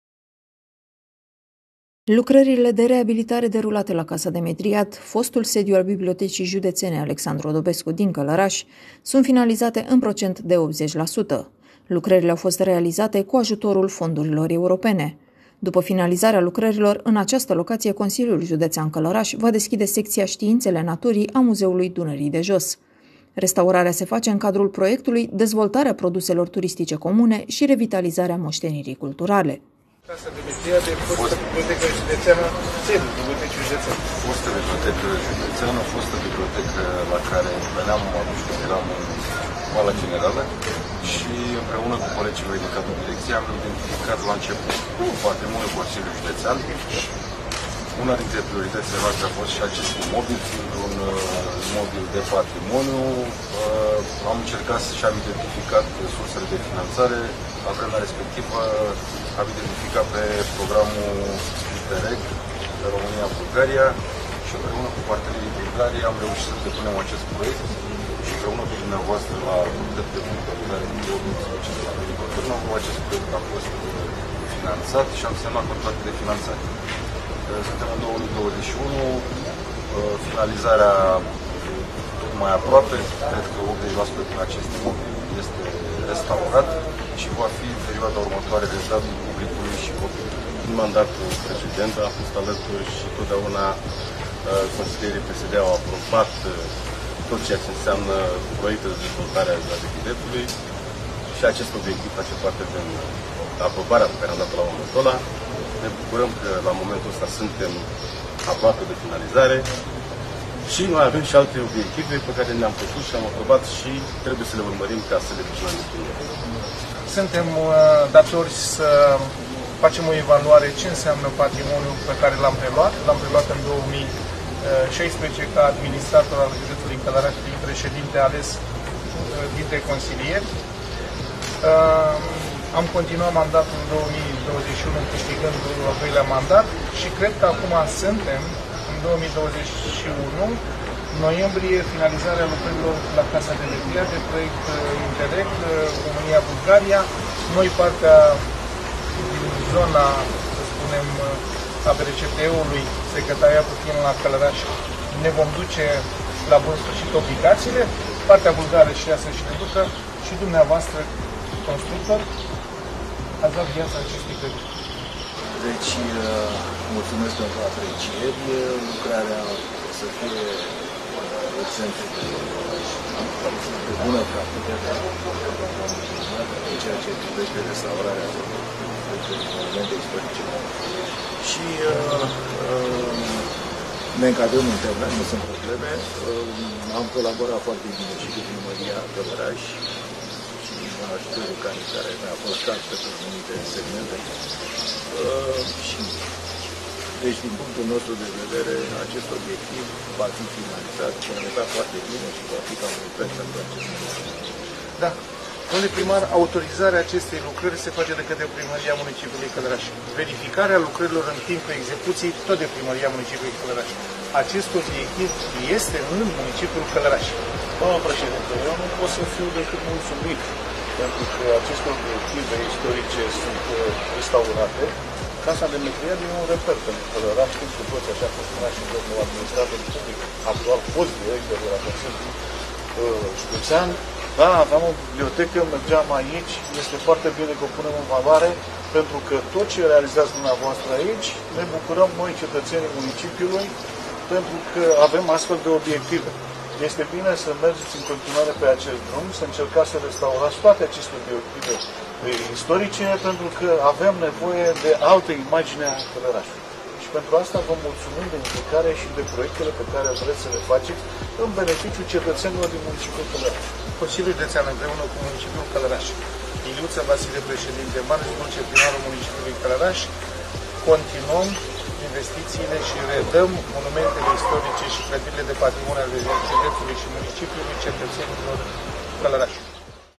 Casa Demetriad, aproape de finalizarea lucrărilor
Restaurarea se face în cadrul proiectului "Dezvoltarea produselor turistice comune şi revitalizarea moştenirii culturale".